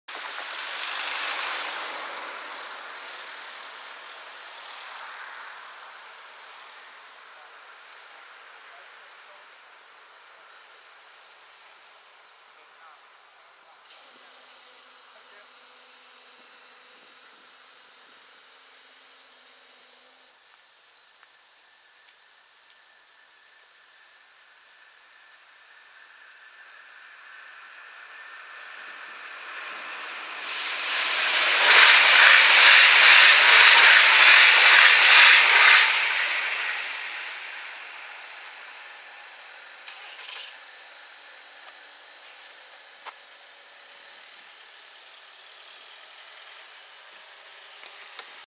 Spoorwegovergang Osterbrock (D) // Railroad crossing // Bahnübergang
Locatie: Königstraße
Traject: Rheine - Leer
Rode lichten: 5
Bomen: 2
Andreaskruisen: 5
Passeren:
-FLIRT als RE15 Emden →Münster